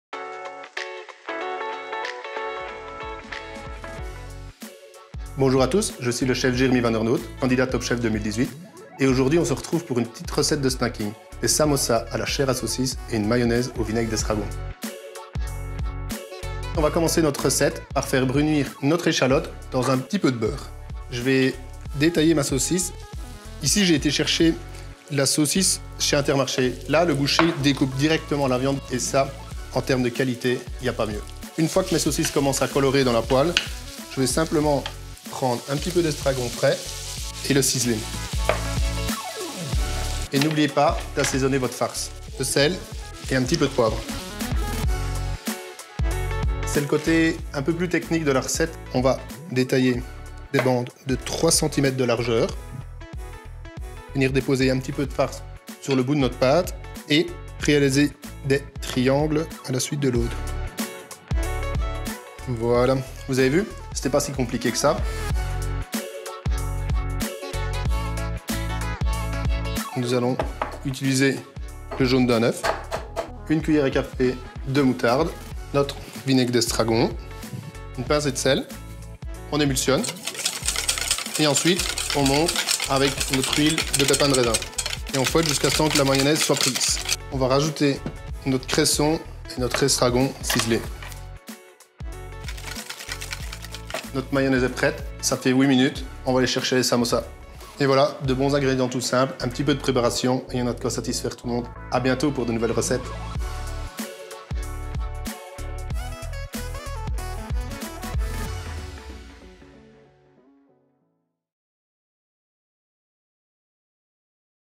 Samossas de chair à saucisses par Jeremy Vandernoot
Voici une recette parfaite en entrée ou en apéritif !

Le chef Jeremy Vandernoot ajoute une touche d'originalité à ses samossas avec une mayonnaise bien de saison.

Bon appétit !